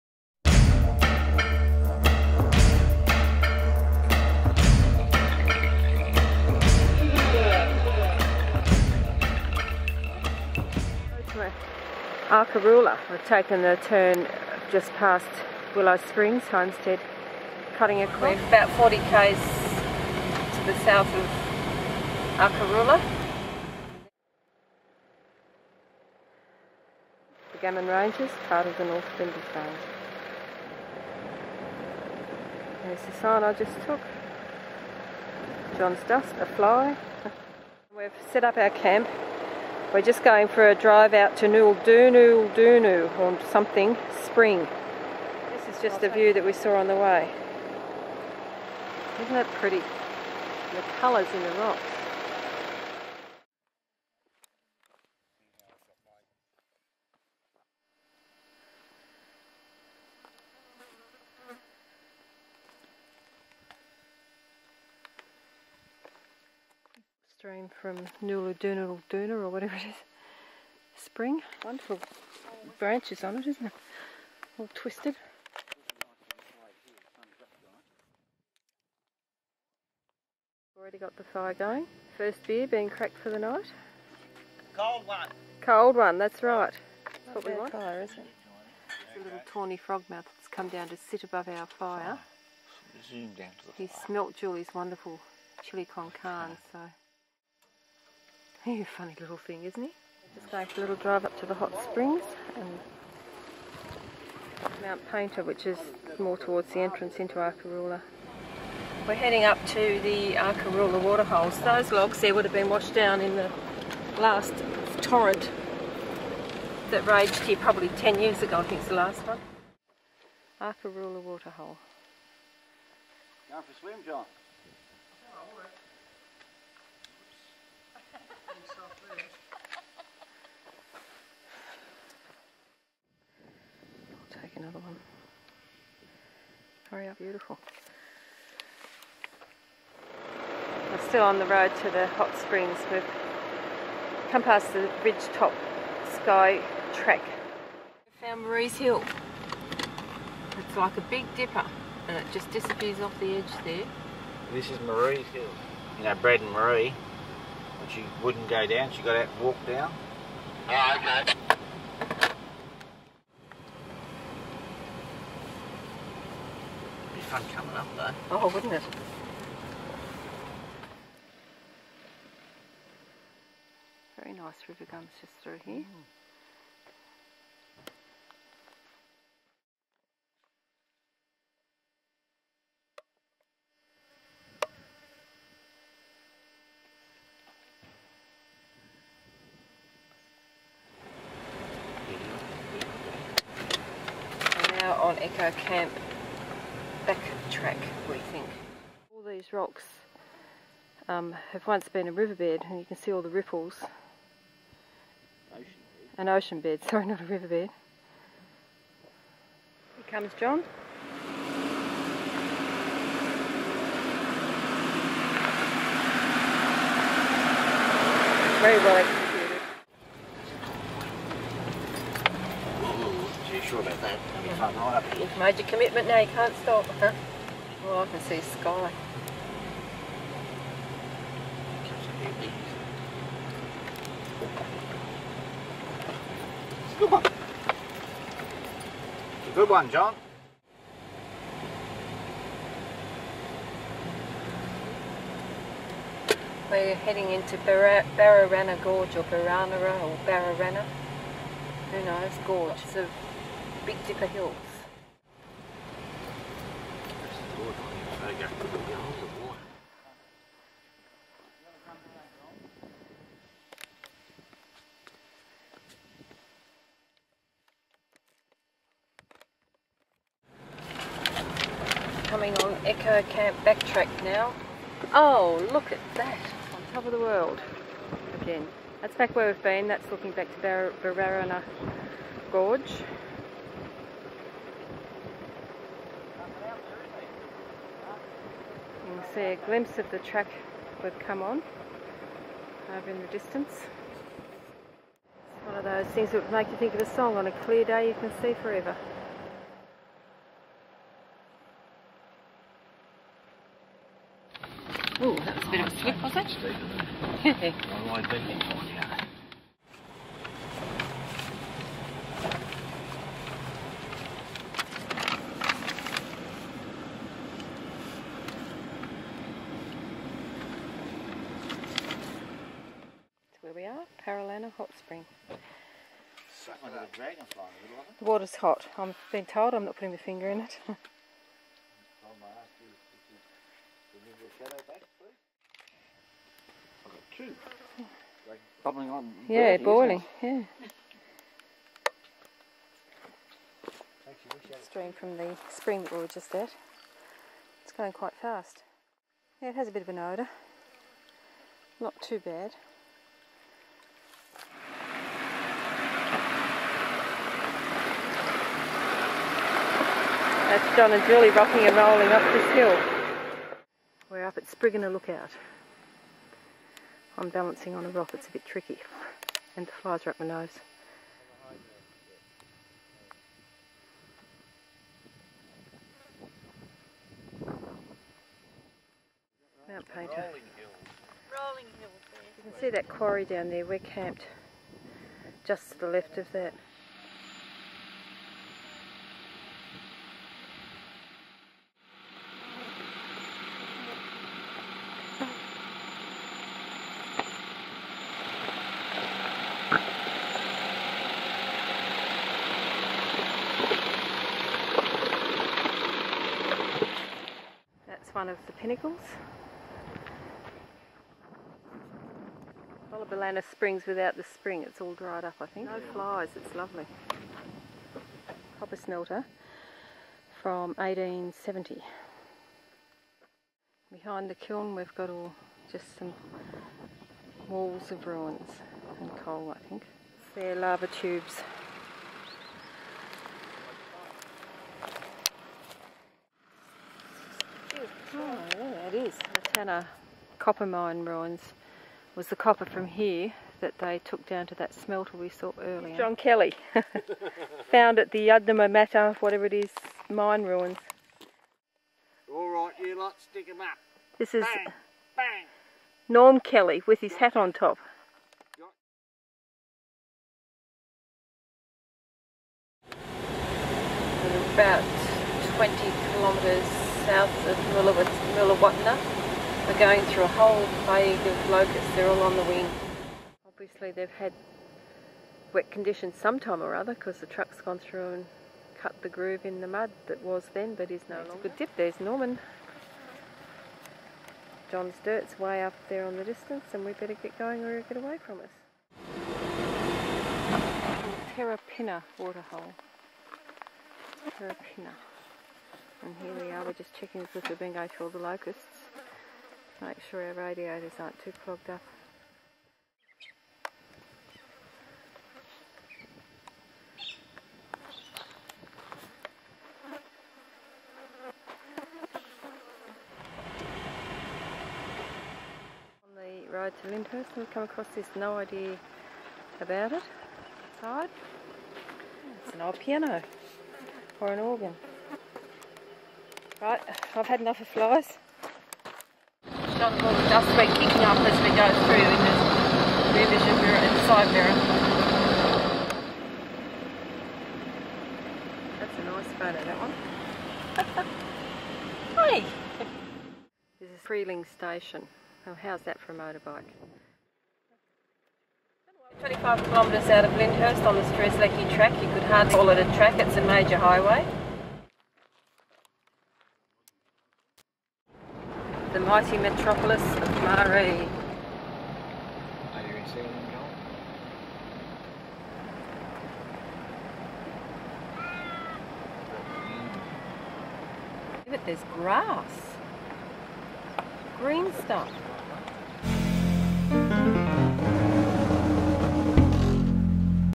2004 003 ARKAROOLA
Exploring Arkaroola then drive  up to Maree